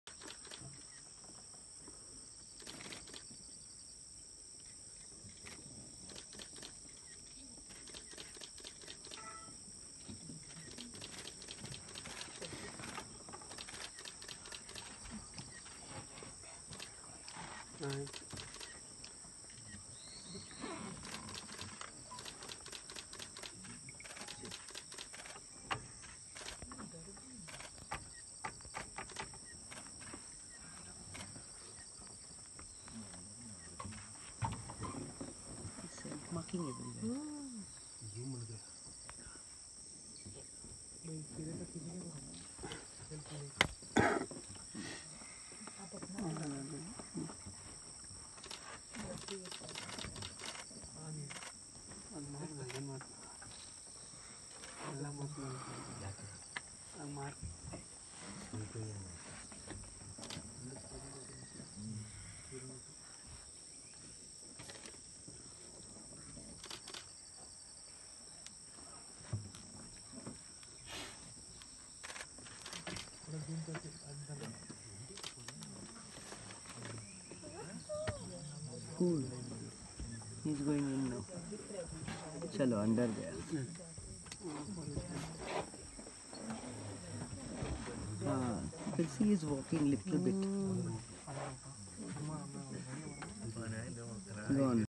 Tiger (Bengal Tiger) Spotting at Mudumalai , India | இந்தியாவின் முதுமலை தேசிய பூங்காவில் புலி
Tiger spotting at Mudumalai National Park, India